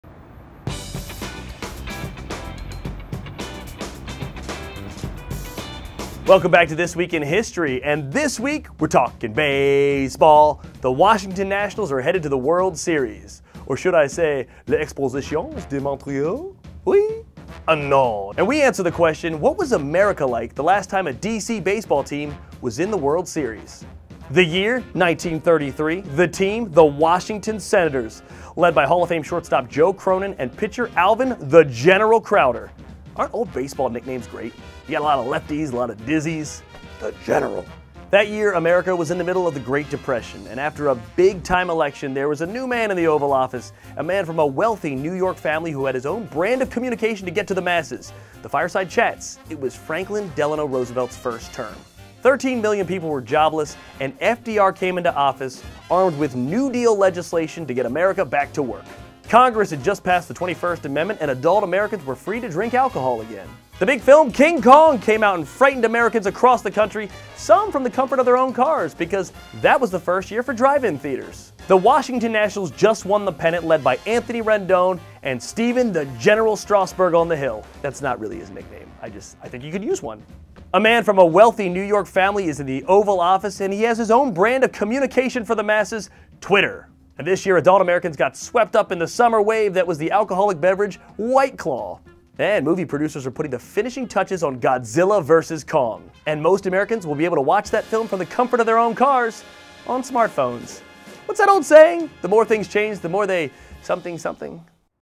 This Week in History: What was America like the last time DC was in a World Series?
The year? 1933. The team? The Washington Senators. That year, America was in the middle of the Great Depression.